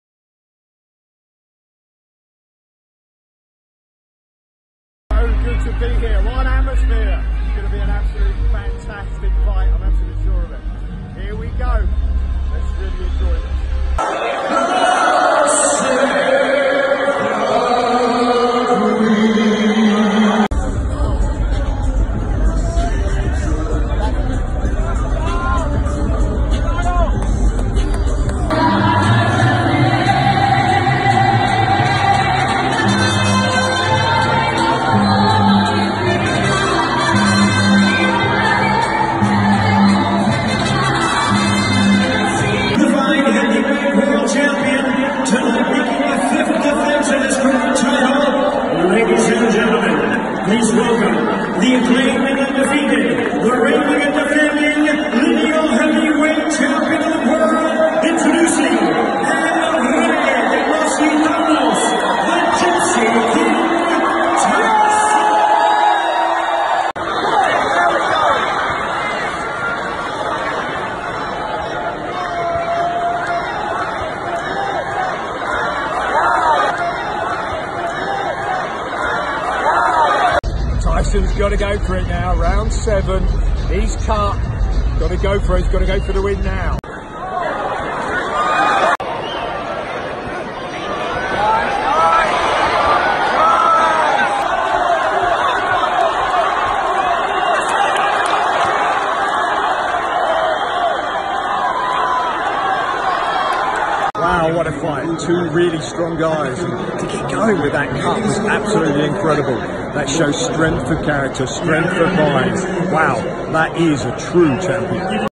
Clips Ringside Las Vegas Tyson Fury vs Otto Wallin inc Mexican Entrance! Nigel Green deVere
Highlights of Tyson Fury vs Otto Wallin, what a fantastic evening of boxing 🥊
Two undefeated fighters in Tyson Fury and Otto Wallin came up against each other at the T-Mobile centre in Las Vegas. Tyson Fury was defending his lineal heavyweight title against Otto Wallin whilst both men challenged for the WBC Mayan belt on Mexican Independance weekend. This was the Gypsy King's second fight since his controversial draw with Deontay Wilder. Fury is set to rematch Wilder on February 22nd 2020.